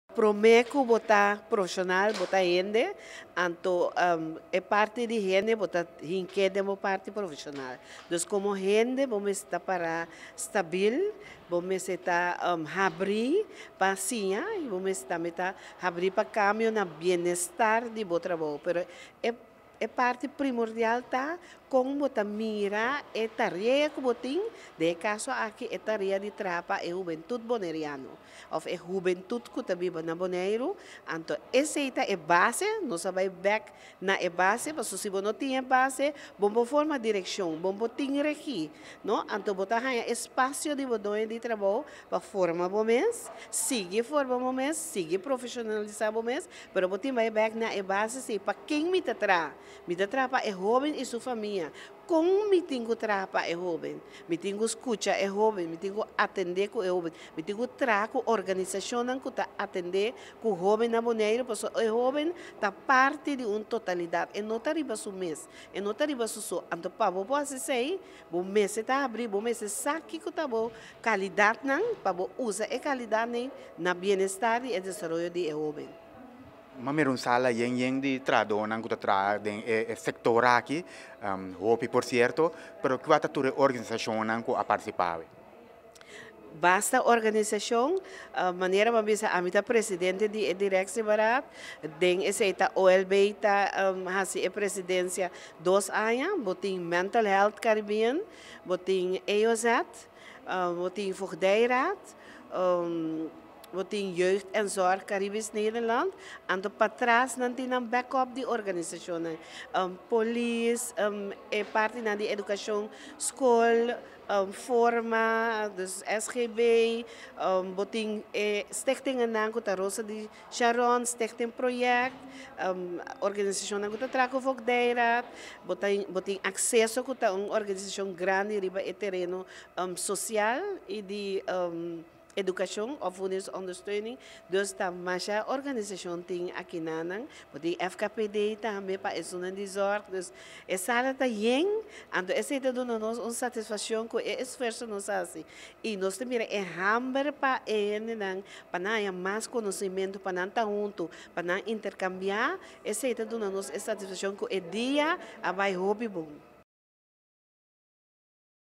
Dag van de Jeugdprofessionals 3 | Sylvana Janga Serfilia
Durante di e ‘Dia di profeshonalnan riba tereno di hubentut’ señora Serfilia a mustra ku pa profeshonalnan kompronde e hoben i su nesesidatnan nan mester traha riba nan mes na prome luga.